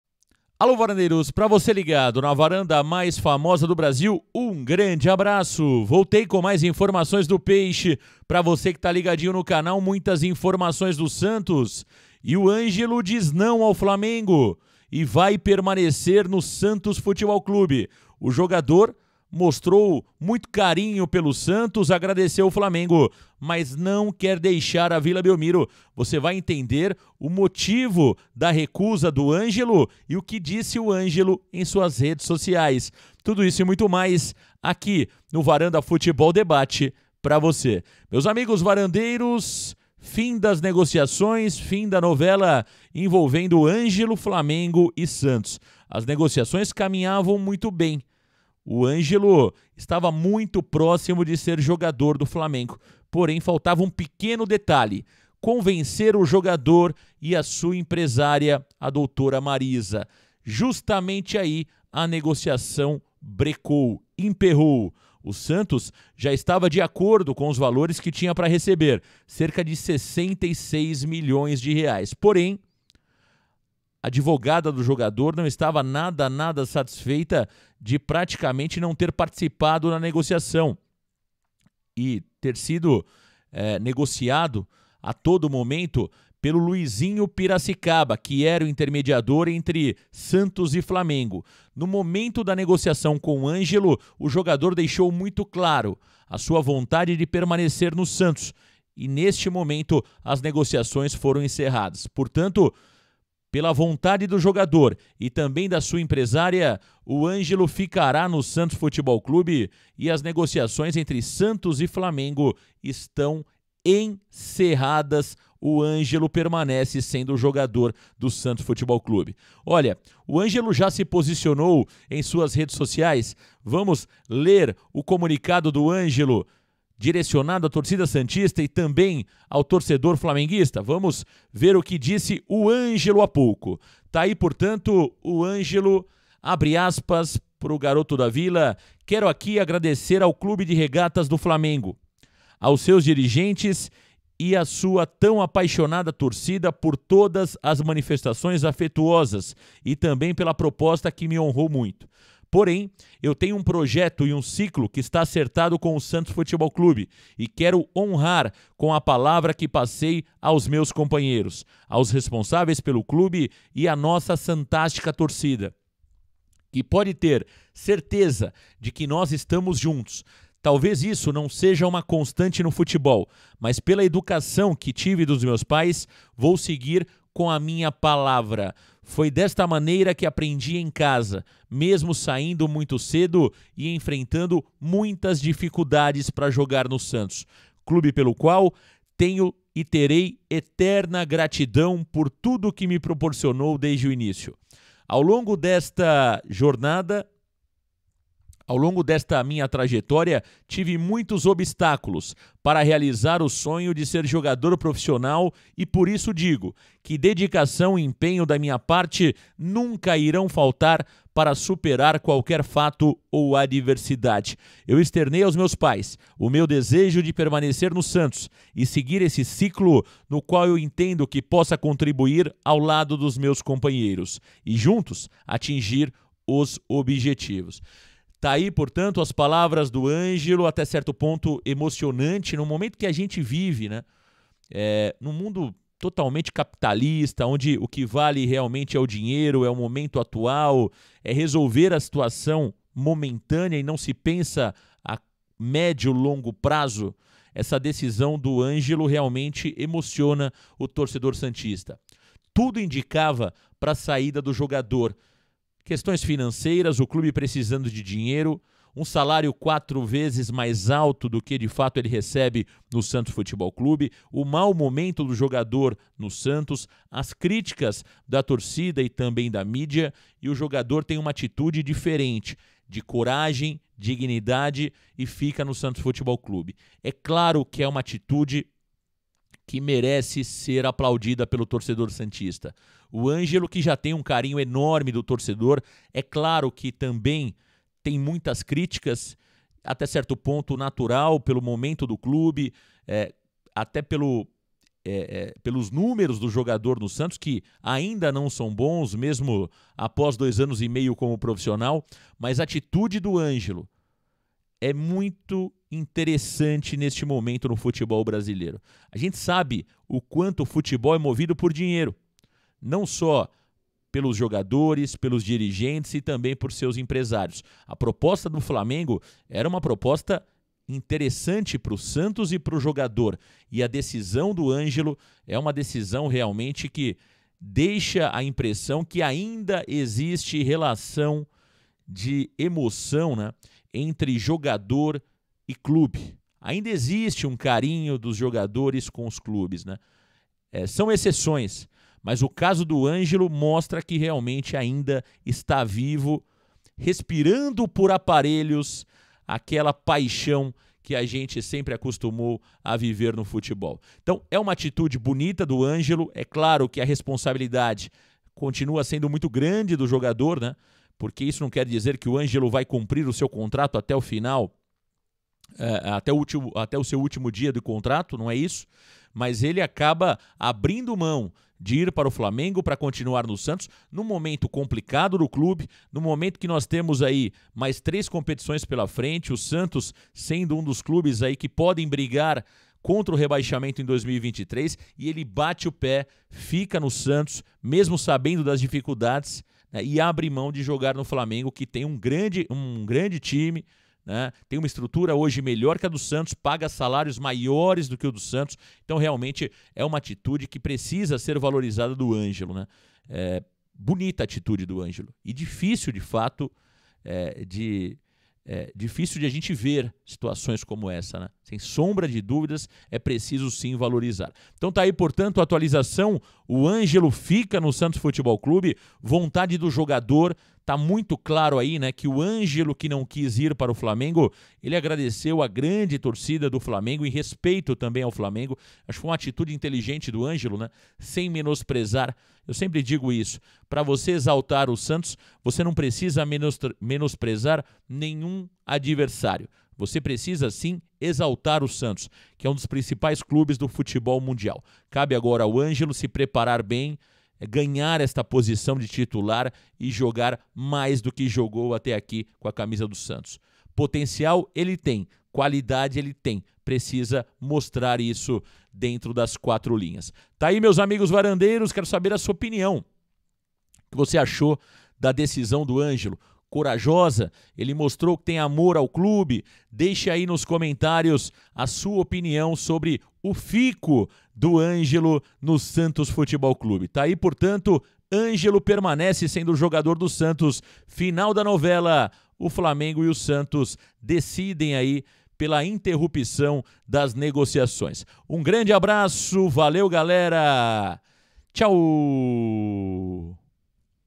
NAÇÃO SANTISTA "EU FICO" | FIM DA NOVELA, ÂNGELO FICA NO SANTOS
Todas as Vinhetas por:
Sonoplastia e Finalização: Gabriel ''GM'' Garcia
Locução: André Cosmo